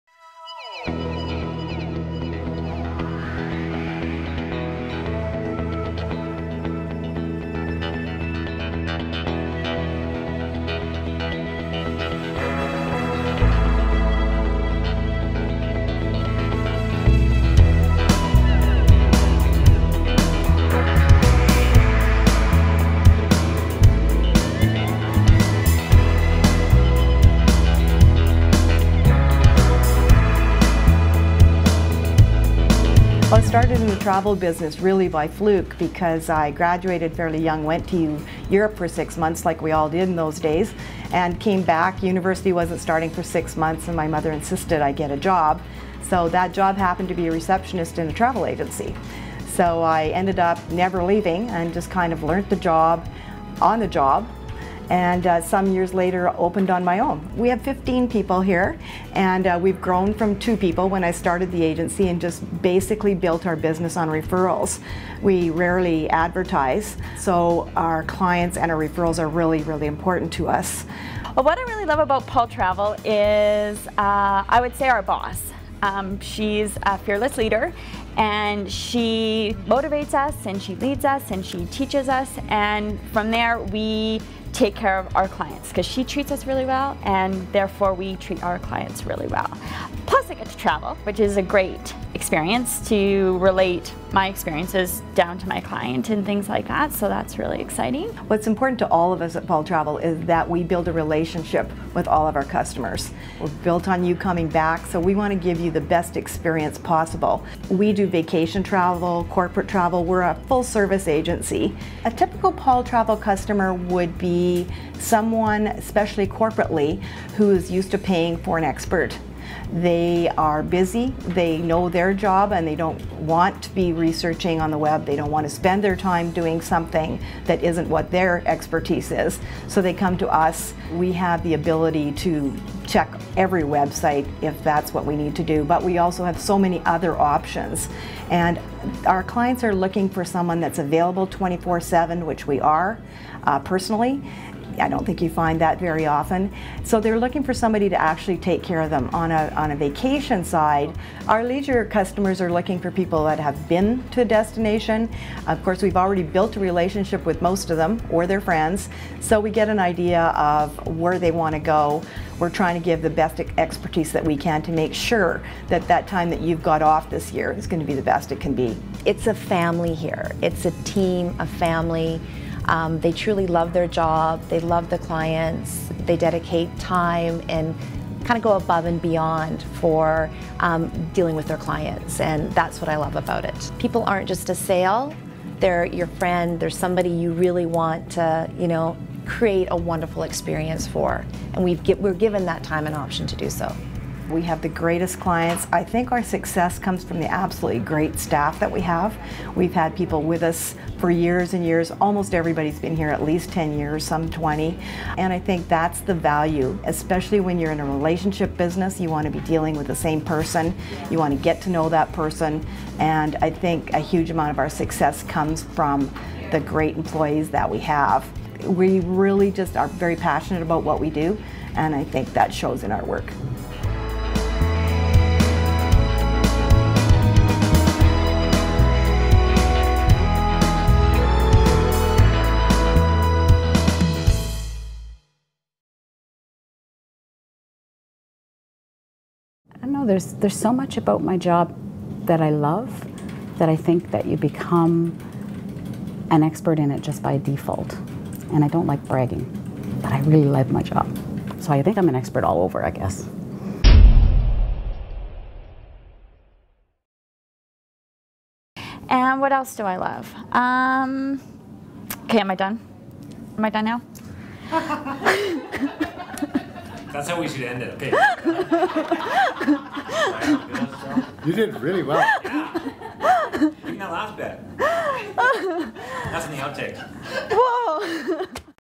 Paull Travel | Edmonton Travel Agency
Paull Travel is locally owned and operated by Lesley Paull who started the agency in 1986 after working as a consultant in Edmonton for 15 years. Our goal is to provide complete service, and take care of all your travel needs.